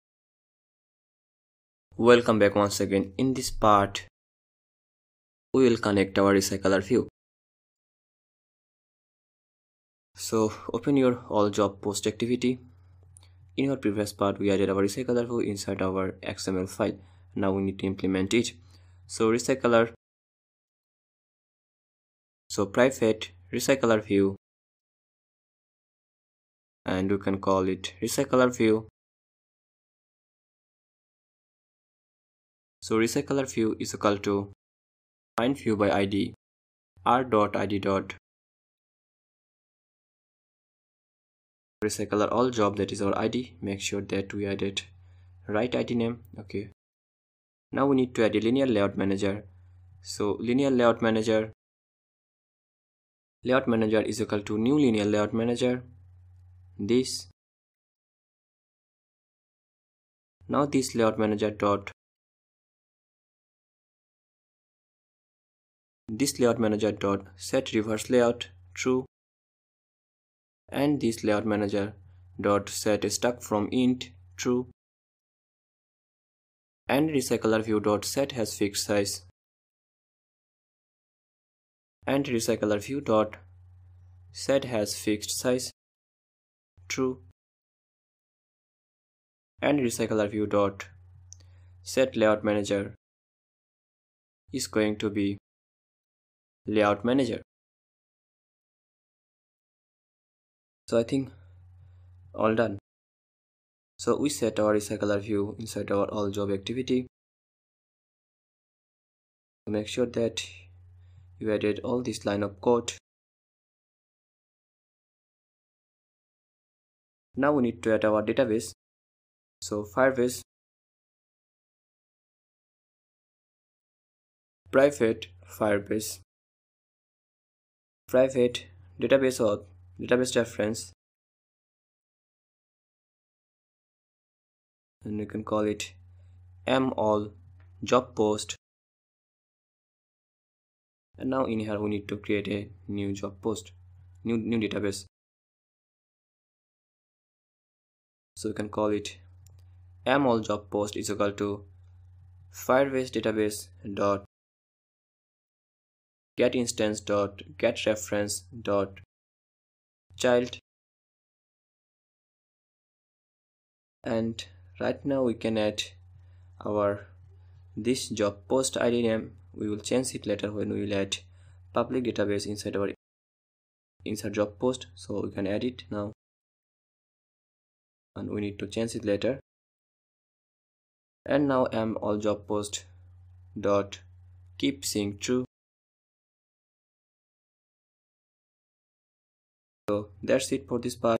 Job Portal App - Part 30 | Connect RecyclerView for all Job Post
In this tutorial, we will learn how toConnect RecyclerView for all Job Post